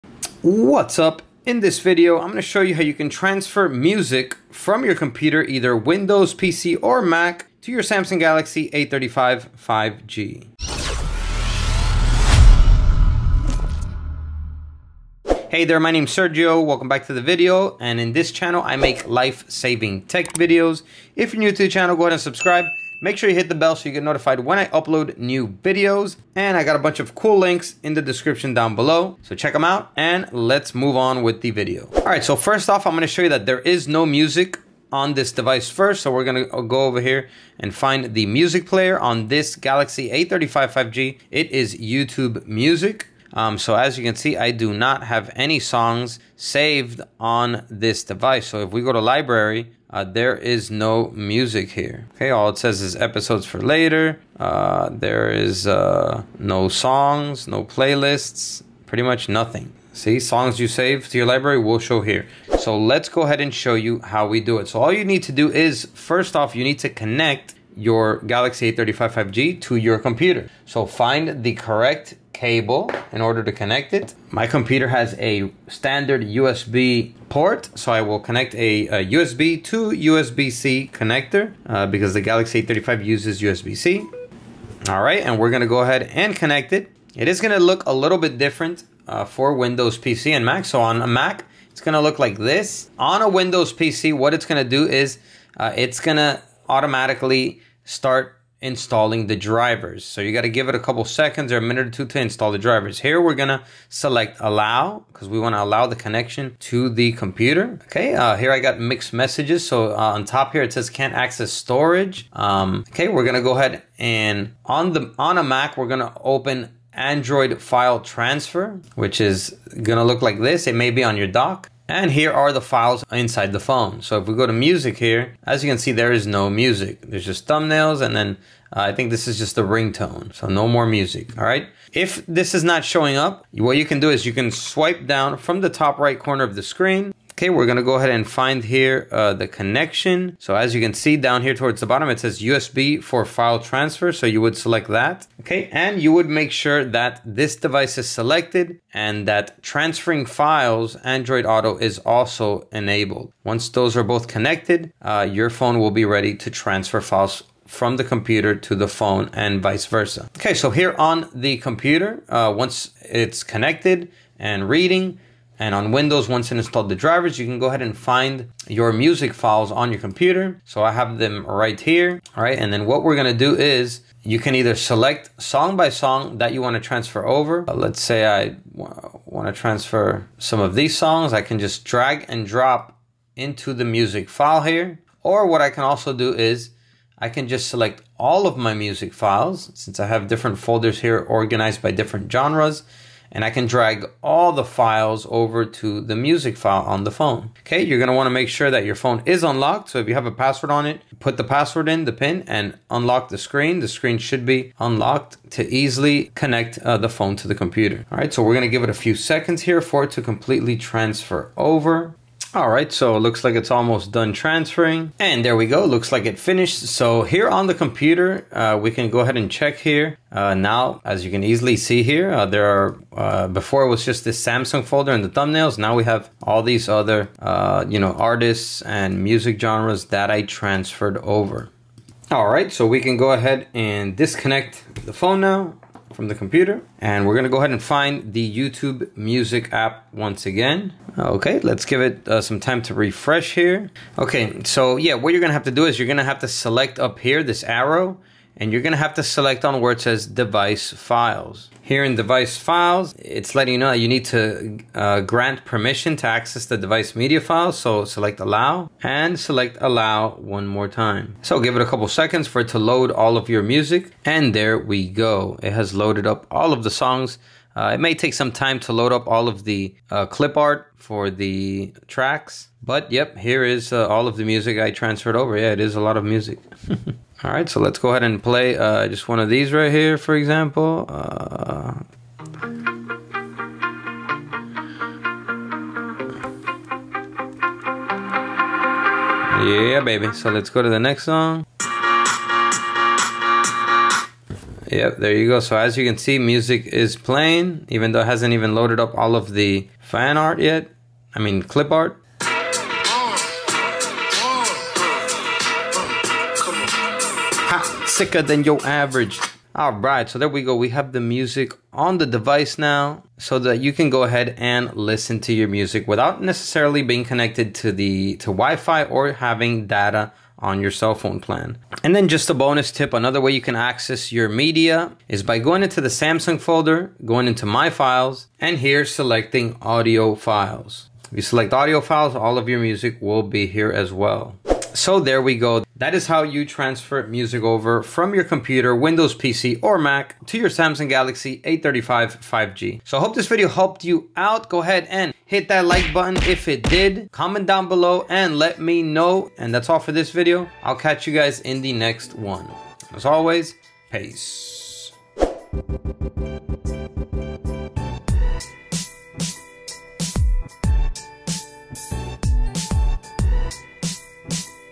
How To Transfer Music From Computer To Samsung Galaxy A35 5G
Do you want to be able to listen to music from your Galaxy A35 without having to be connected to Wifi or Data?

Well now you can! In this video I show you how to transfer music from your MacBook or PC computer to your Samsung Galaxy A35 5G so that you can listen to music directly on the go and without the need to connect to the internet.

📱Cool Tech
iPhone Parts
Repair Tools
Best MacBook Accessory
Unlock Your iPhone